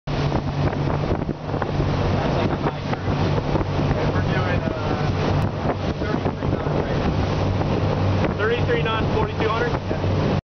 33kts. Cruising Speed.MOV
2800 Kevlacat with Twin 250HP Susuki Four Stroke Outboards. Sea Trial, on the water Boat Test by Capt. Adam Hermsen of Breakwater Yachts, Oceanside, California Dec. 14, 2011. Video, Cruising Speed / 4200 RPMS / Speed @ 33kts.